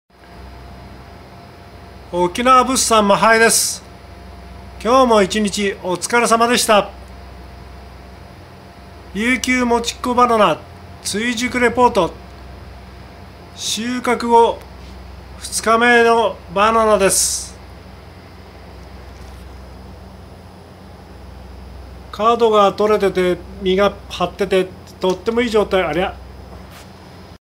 2021年7月1 夕方、 琉球もちっ娘バナナ 追熟レポート 編その1、 収穫後、2日目。 沖縄物産 真南風 より (埼玉県川越市 大正浪漫夢通り )
今日も1日お疲れ様でしたシリーズ、琉球もちっ娘バナナ 追熟編、その１
（7/1～7)。
収穫後二日目。
川越も気温が高くなっているので青みが抜けるのが早いです。
まだ、果物というより植物というようなつんつんした香りです。